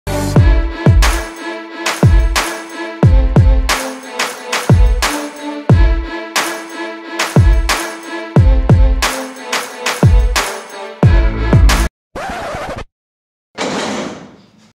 School freestyle fail
I tried to film a freestyle demo at my school but it didn't work out :)).